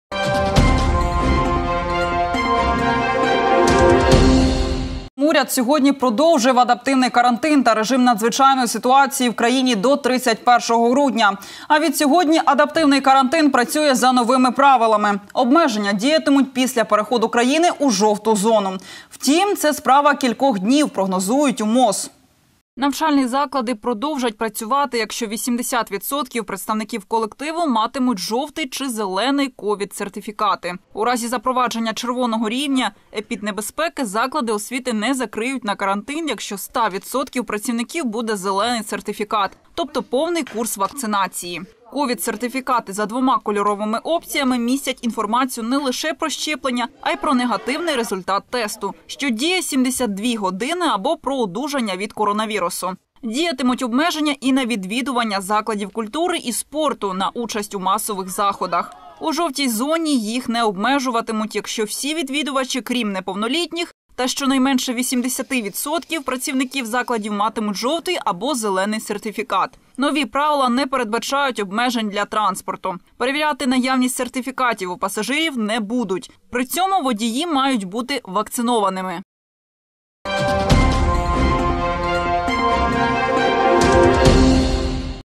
З 20-го вересня в Україні починають діяти нові карантинні норми
Уряд сьогодні продовжив адаптивний карантин та режим надзвичайної ситуації в країні до 31 грудня. А відсьогодні адаптивний карантин працює за новими правилами. Обмеження діятимуть після переходу країни у "жовту" зону. Втім, це - справа кількох днів, прогнозують у МОЗ.